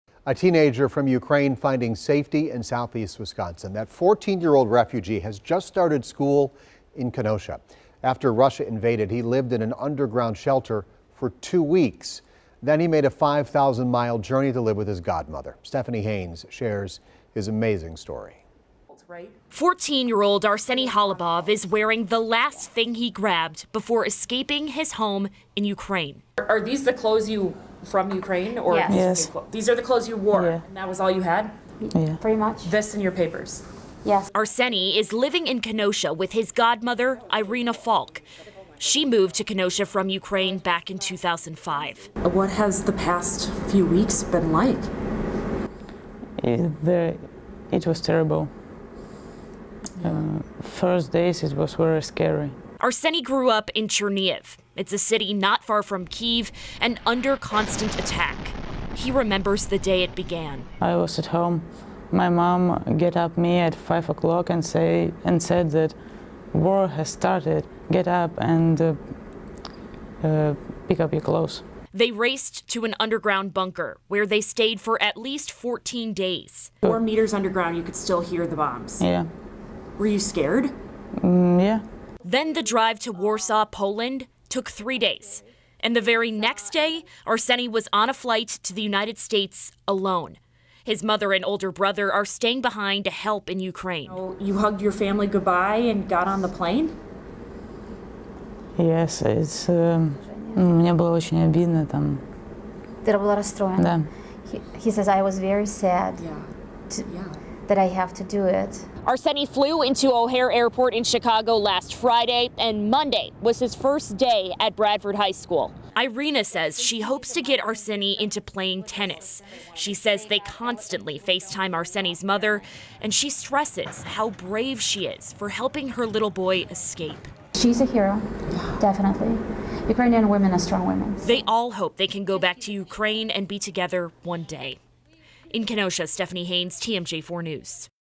Ukrainian teenager escapes war to live in Kenosha
A Ukrainian teenager escaped the war and is now living with his godmother in Kenosha.